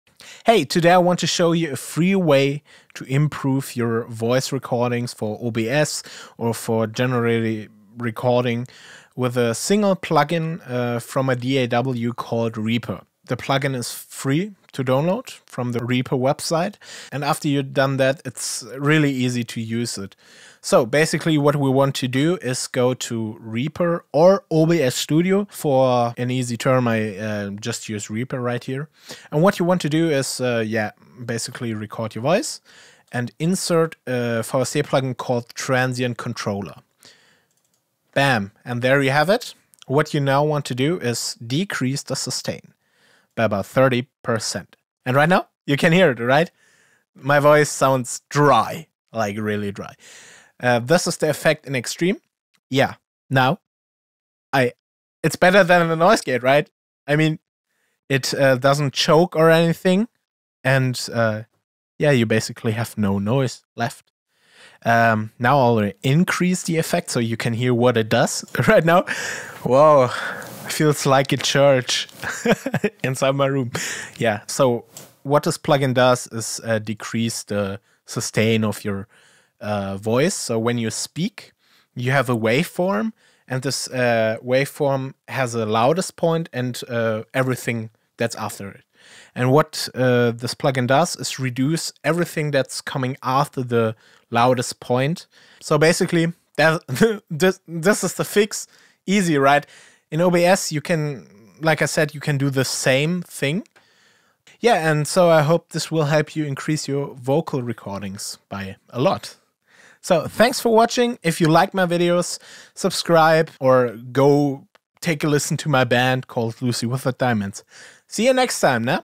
How to: Reduce Room Echo and Noise WITHOUT a Noise-Gate (OBS Studio, Reaper)
Hey!
You can reduce room echo and noise like a Computer fan with a simple vst Plugin type called "Transient Controller" from the VST plugin pack inside the 60 day trial from the DAW called Reaper from Cockos.

You can also use this Plugin inside OBS-Studio or OBS Streamlabs with the filter function. Just use the "VST‐2.x‐Plugin" Filter and load the plugin.

A Transient Designer is a helpful tool when it comes to reducing room-sound from a source.
It also works different than a noise gate because it doesn´t just cut out the signal and muting it but reduces the background noise for the whole time.

If you want to pay for one, I can recommend Fabfilter Saturn next to this one.

Cannot find the plugin inside OBS?
You have to install the plugins into one of these directions to work:

Windows (*.dll)
C:/Program Files/Steinberg/VstPlugins/
C:/Program Files/Common Files/Steinberg/Shared Components/
C:/Program Files/Common Files/VST2
C:/Program Files/Common Files/VSTPlugins/
C:/Program Files/VSTPlugins/
macOS (*.vst)
/Library/Audio/Plug-Ins/VST/
~/Library/Audio/Plug-ins/VST/

►  CAMERA:   *Sony Alpha 6000 into Elgato Camlink*
►  LIGHTS:  *IKEA Tertia*

Timestamps:
00:00 Intro
00:21 Which Plugin is it?
00:40 What to do with it
01:15 The way the Plugin works
01:06 Why it works
01:43 How to use it in OBS
01:46 Outro